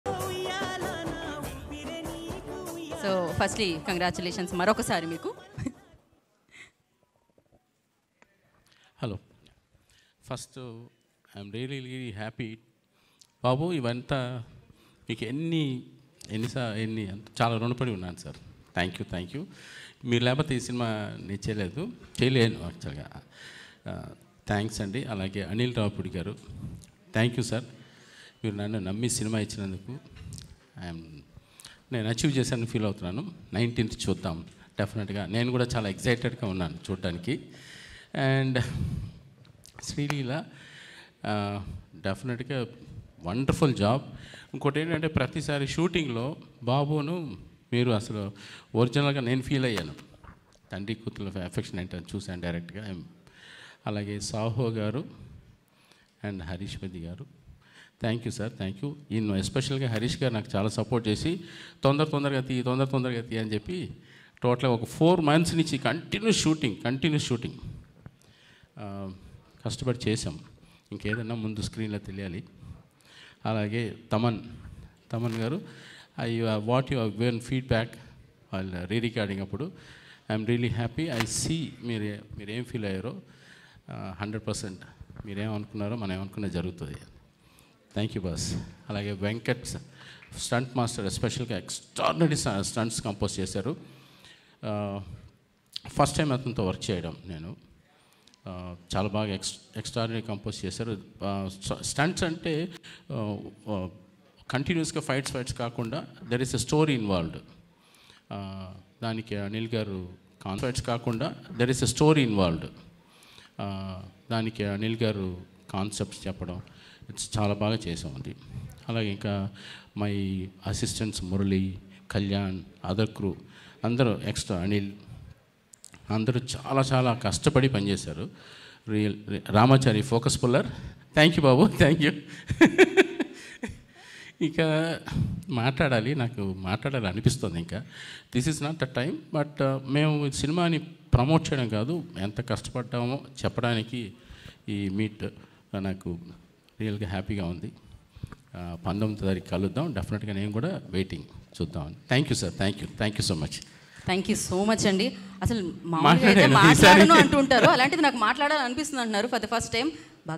DOP Ramprasad Speech | Bhagavanth Kesari Grand Press Meet | Nandamuri Balakrishna | Anil Ravipudi
DOP Ramprasad Speech, Bhagavanth Kesari Grand Press Meet On Shine Screens.

Bhagavanth Kesari 2023 Latest Telugu Movie starring Nandamuri Balakrishna, Kajal Aggarwal, Sreeleela & others. Directed by Anil Ravipudi, Music composed by S Thaman, Produced by Harish Peddi & Sahu Garapati Under Shine Screens Banner.

Movie Details:

Movie Name: Bhagavanth Kesari
Cast: Nandamuri Balakrishna, Kajal Aggarwal, Sreeleela, Arjun Rampal & others
Director: Anil Ravipudi
Producer: Harish Peddi & Sahu Garapati
Banner: Shine Screens
Marketing : Haashtag Media, First Show